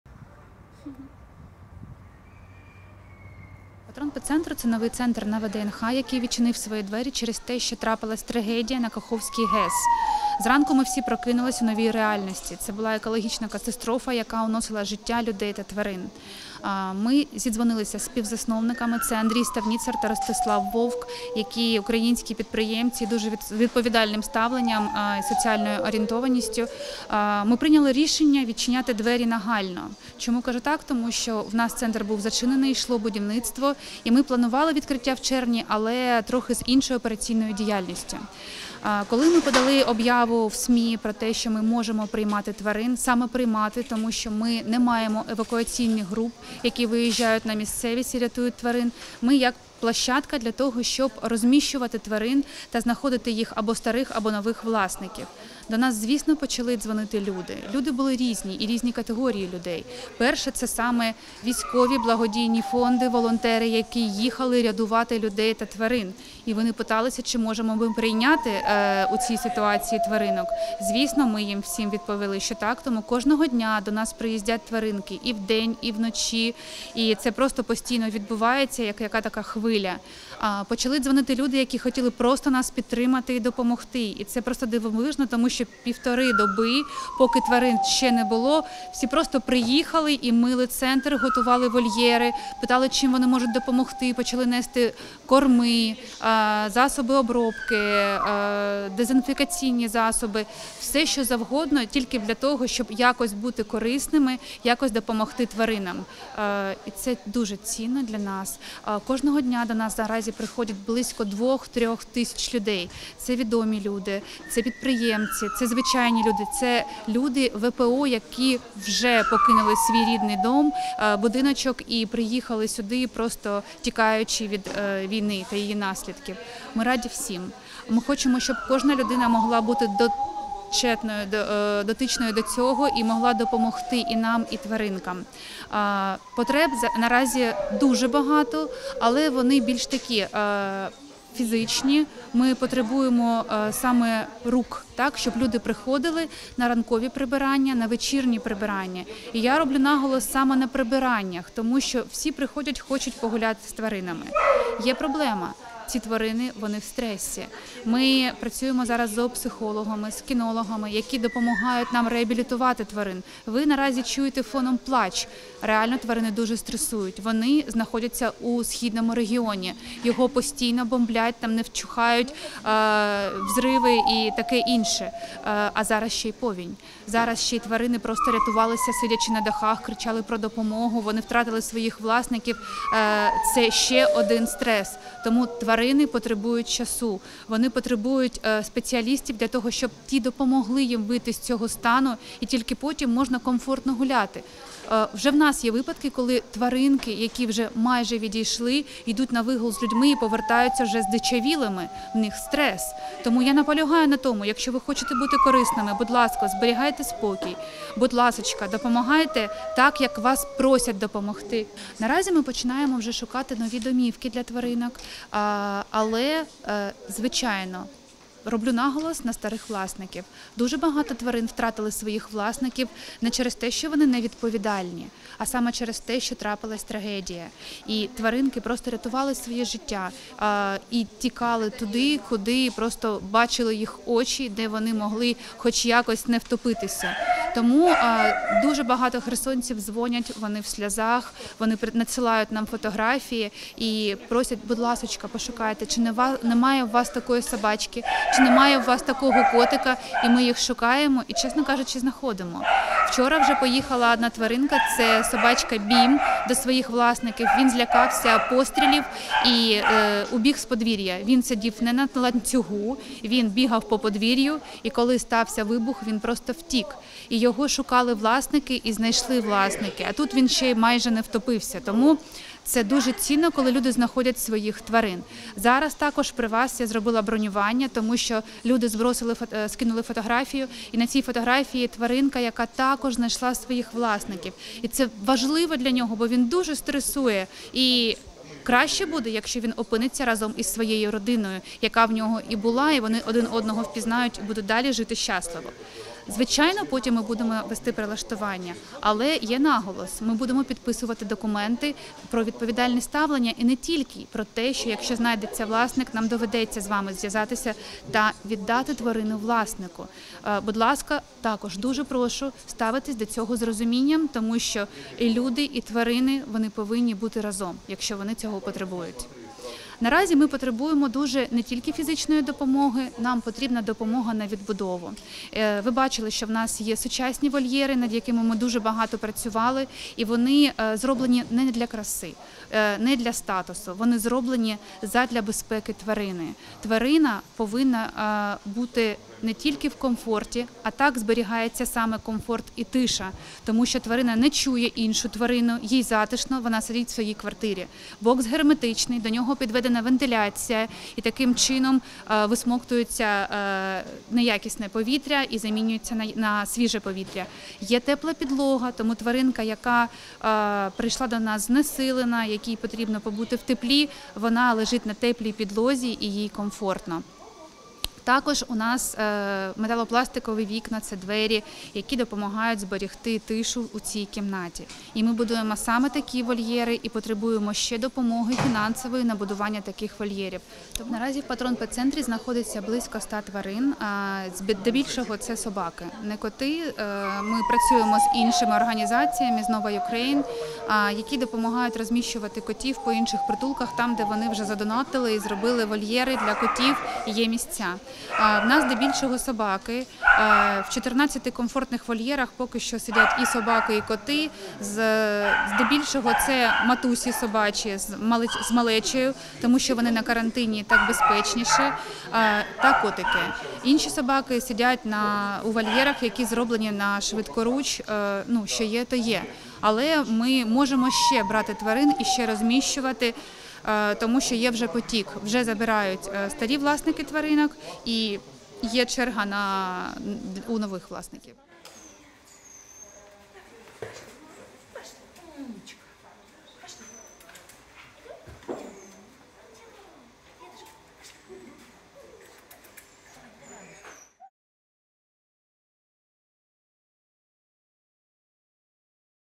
У Patron Pet center відкрили зоохаб для тварин з Херсону
У Києві у Patron Pet center на ВДНГ відкрили волонтерський зоозахисний хаб для собак і котів, вивезених з Херсона.

Оскільки у центрі майже всі врятовані тварини - хатні і мають власників, то наразі створюються можливості пошуку власників, які втратили своїх тварин.

Наразі Patron Pet center потребує допомоги «руками», а для вигулу тварин існує спеціальний графік.

Щодо умов у Patron Pet center, то він у процесі будівництва, вольєрів не вистачає, проте тут є 14 нових вольєрів - з теплою підлогою, вентиляцією та герметичними дверима.

відео - Укрінформ